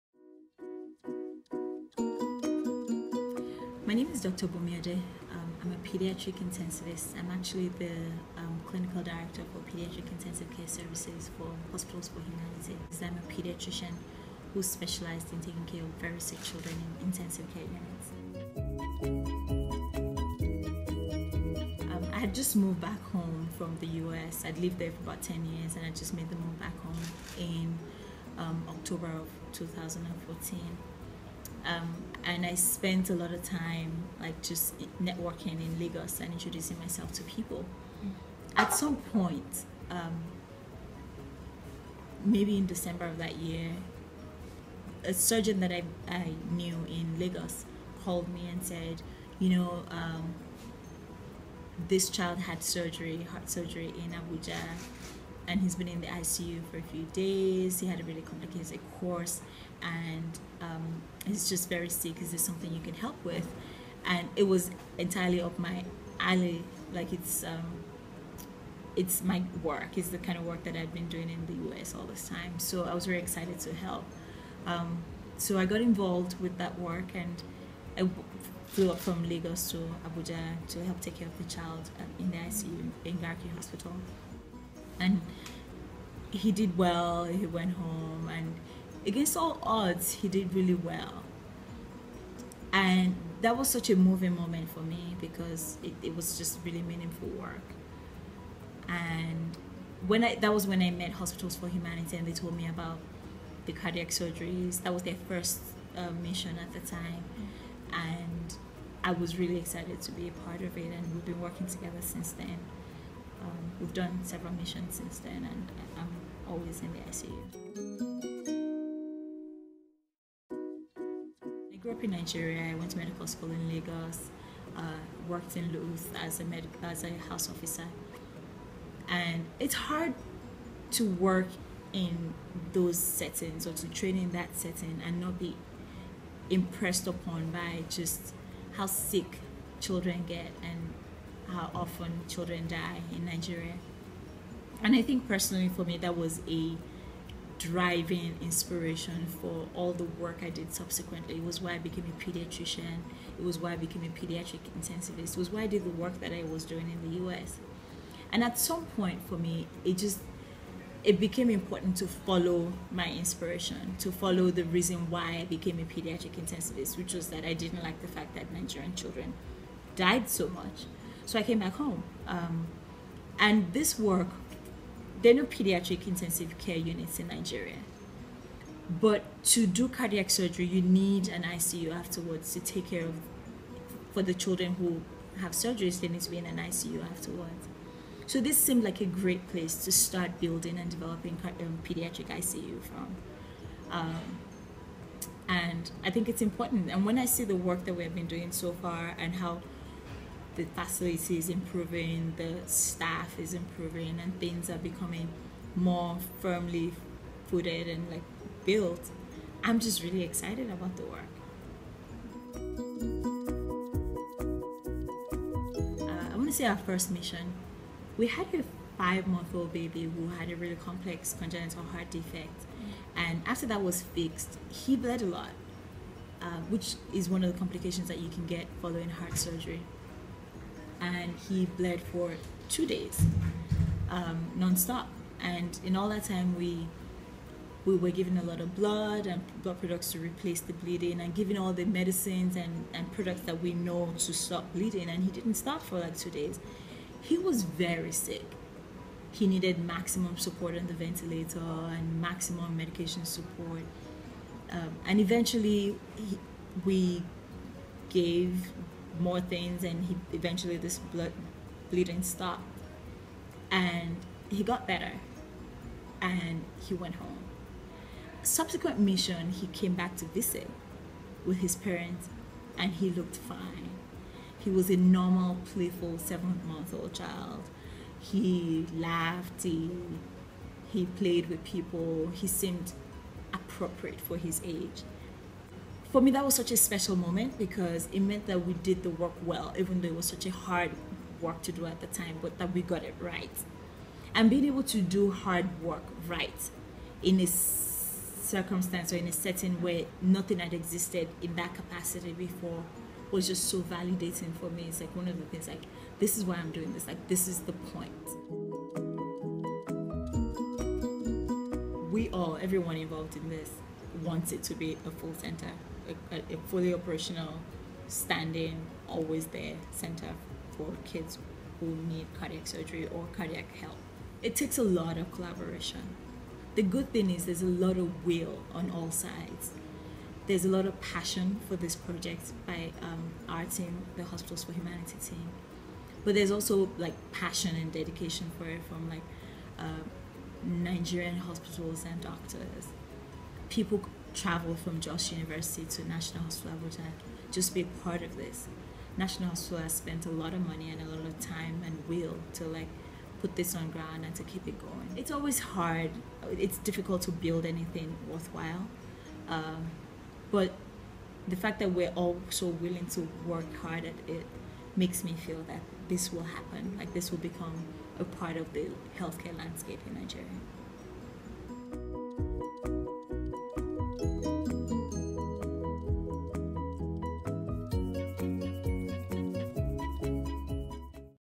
Bunmi's Story: "This is the reason I became a pediatric intensivist"
Dr. Bunmi Ode is a consultant pediatric intensivist. In this Nigeria Health Watch Interview, she highlights an initiative by NGO Hospitals For Humanity (HFH) that is giving back to Nigerian children through performing heart surgeries for congenital heart defects as well as building the capacity of Nigerian doctors, nurses and pharmacists to do the same. She speaks about her own passion to care for children and what she hopes will come out of this initiative.

For more information about this initiative, go to hospitalsforhumanity.org